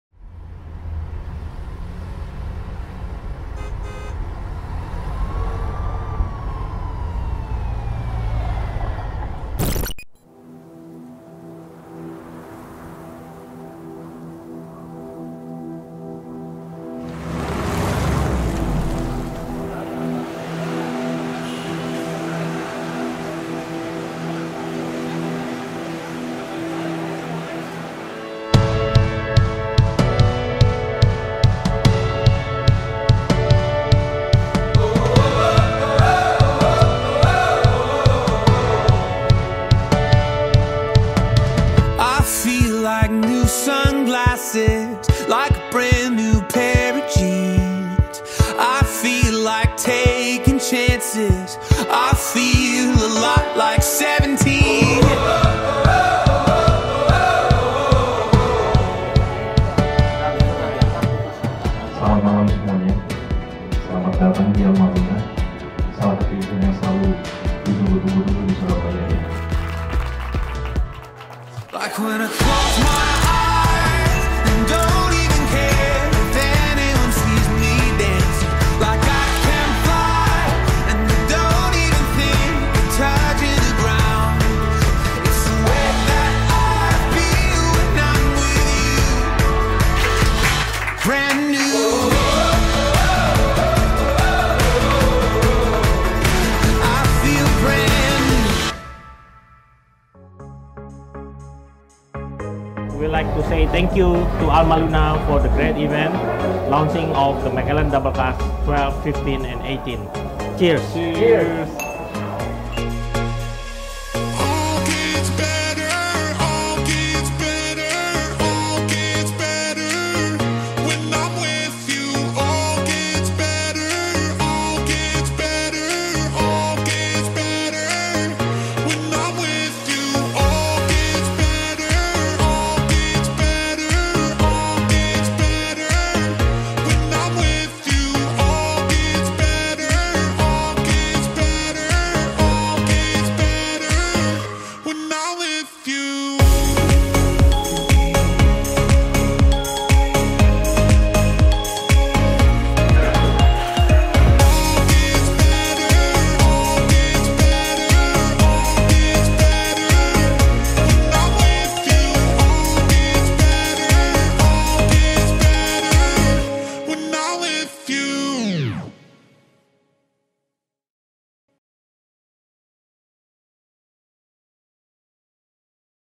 The Macallan Double Cask Launching Party at ALMALUNA Bar & Resto
The global expansion of the Macallan brand continues through The Macallan Double Cask Exceptional; a prestigious event held at ALMALUNA Bar & Resto Surabaya. Proudly introducing the story of Macallan Double Cask Range, the event features an upscale dinner celebration, with live music entertainment by Alderamin Band and DJ Tripleks. Launched in 2016, Double Cask tells an enticing journey that unites two different worlds into one eclectic blend of taste. For years, The Macallan’s Whisky Mastery Team used their skills and expertise to choose the best combination of taste from each oak wood barrel in the United States and Europe to craft the wondrous Double Cask Range.

Crafted without compromise. Please savour The Macallan responsibly.​